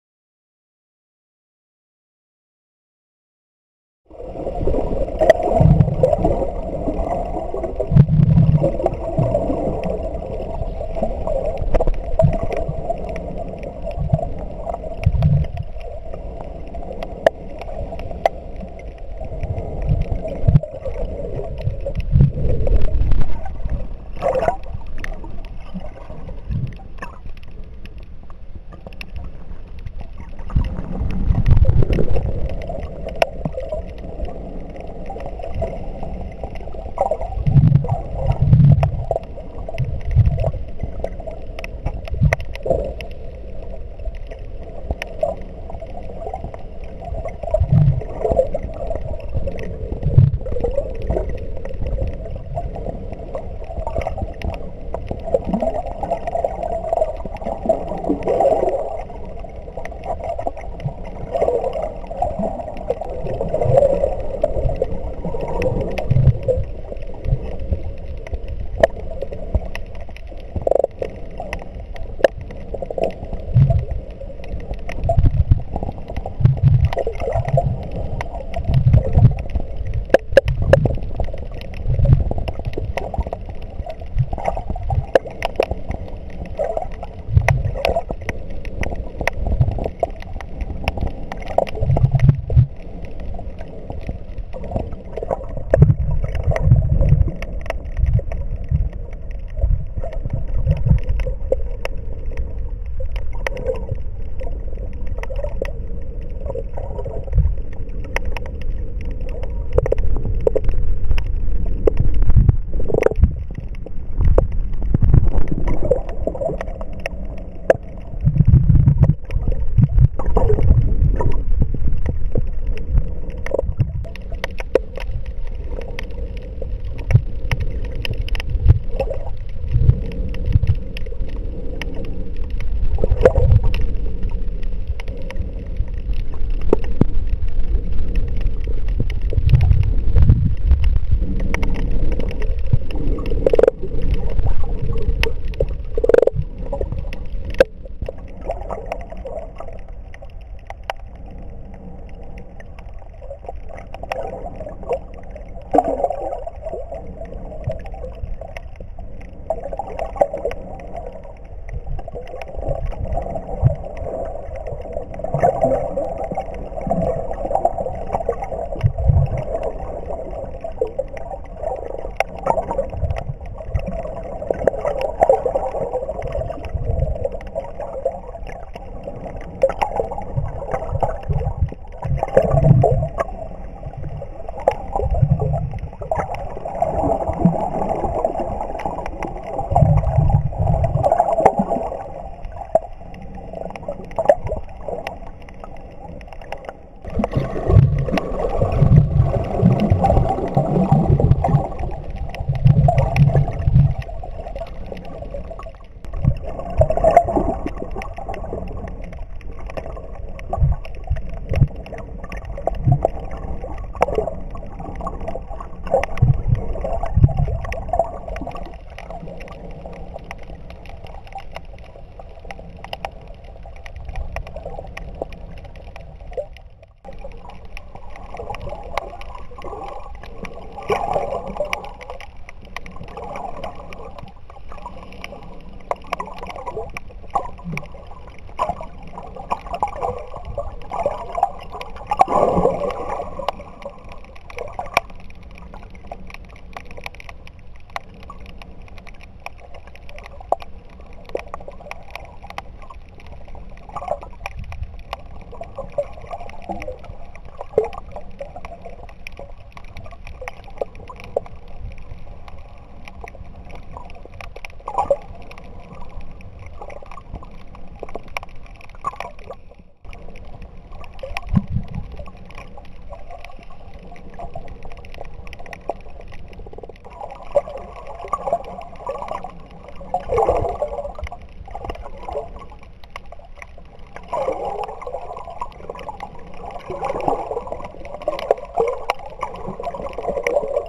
Snorkeling South Friar's Bay, St Kitts
Here is some video footage taken at South Friar's Bay, St Kitts, during a cruise visit in February 2012. Footage taken with a Kodak Easy Share Sport C123 waterproof camera.